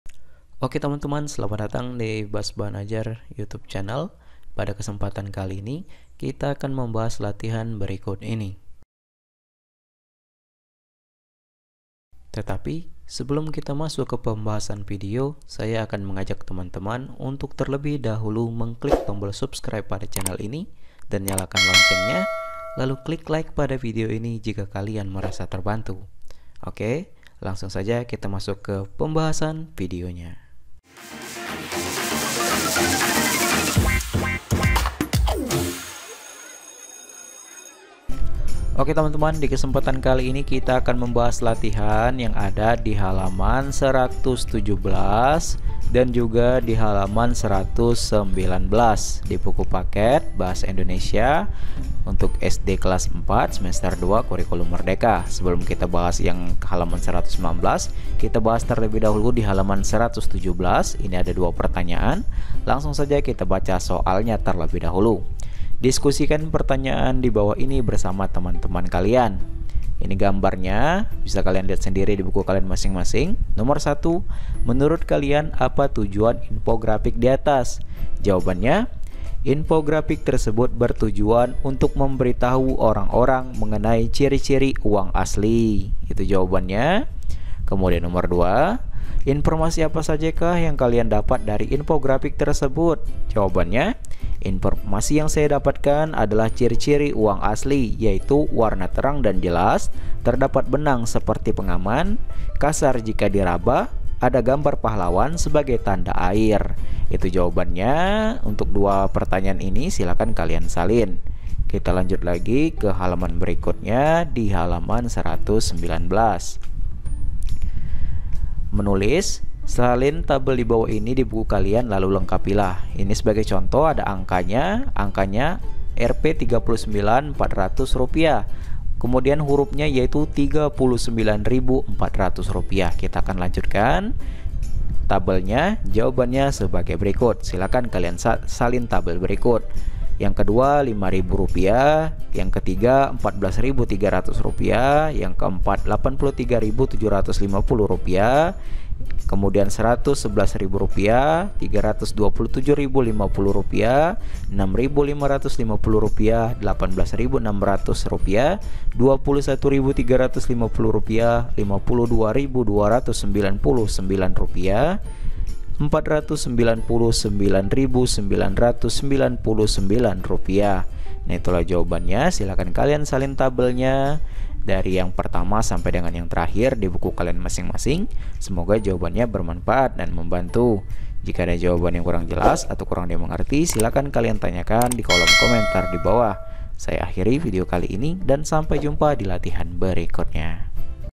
Kunci Jawaban Bahasa Indonesia Kelas 4 Halaman 117 119 Kurikulum Merdeka
Diskusikan pertanyaan di bawah ini bersama teman-teman kalian.
1. Menurut kalian, apa tujuan infografik di atas?
2. Informasi apa sajakah yang kalian dapat dari infografik tersebut?
Sampaikan pendapat kalian secara bergantian. Jika kalian membawa uang, kalian boleh mempraktikkan Dilihat, Diraba, dan Diterawang.